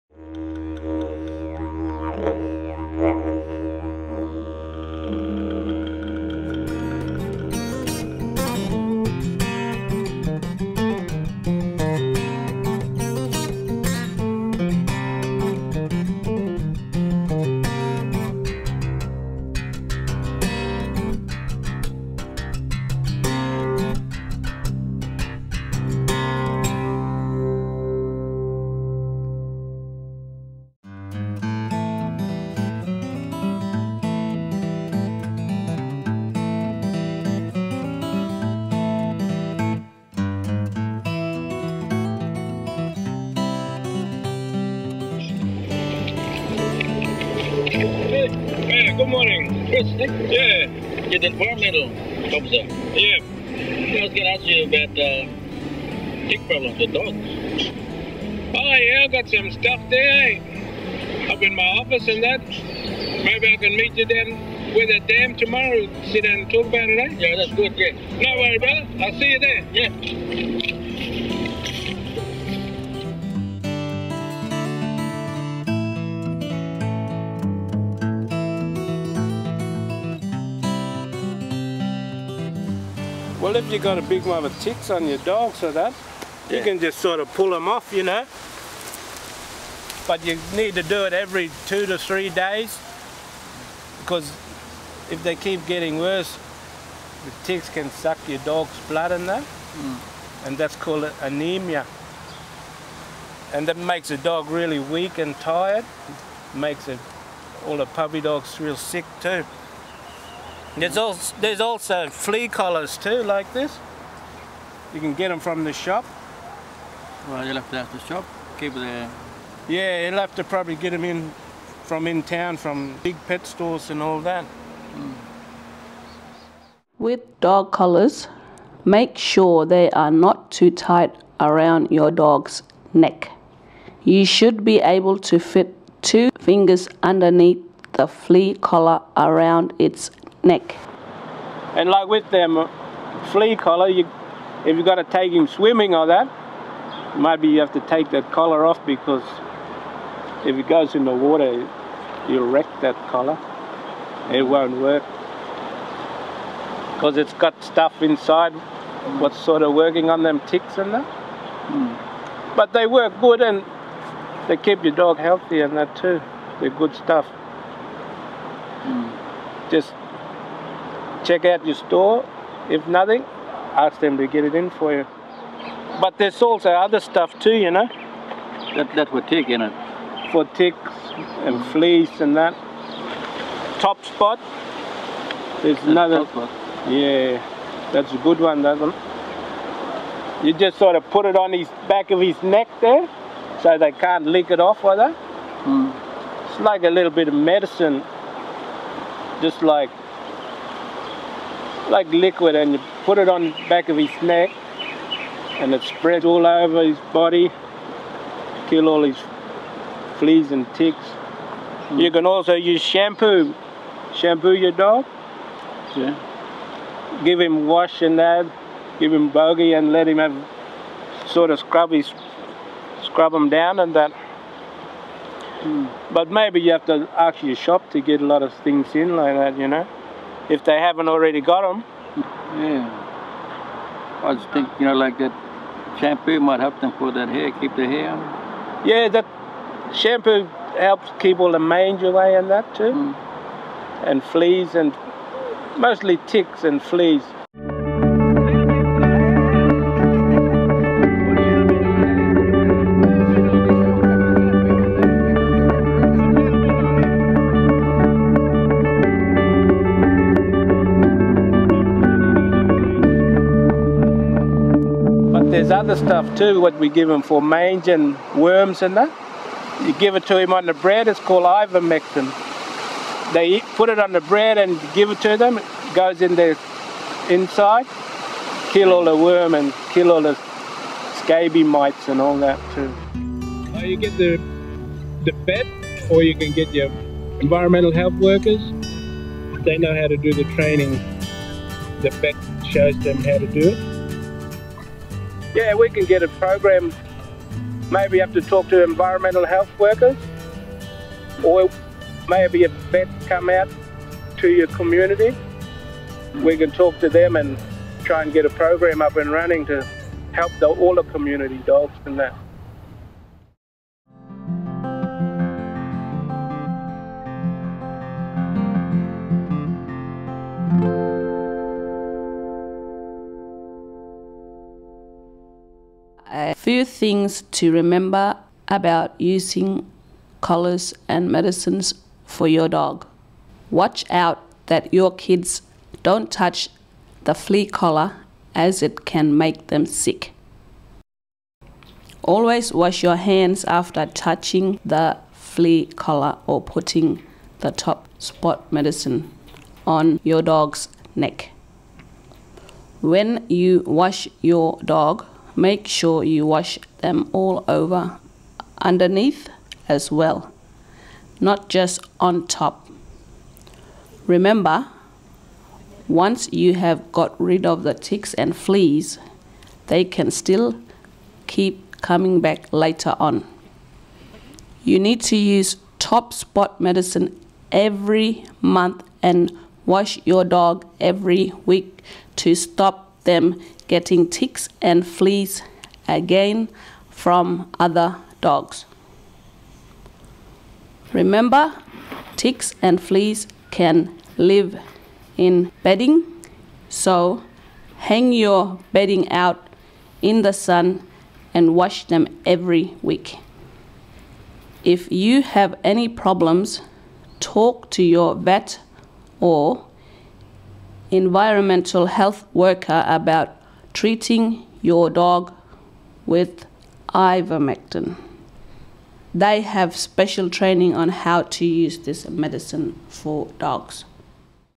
Fleas and Ticks - Caring for Dogs, Community and Country
This is an educational resource movie for Indigenous people in remote communities, it is about treating your dog for ticks and fleas. It contains information on removing ticks, flea collars, flea shampoo and topspot treatments. This video was produced by Animal Management in Rural and Remote Indigenous Communities, it was funded by the Department of Agriculture, Fisheries and Forestry and the Australian Animal Welfare Strategy.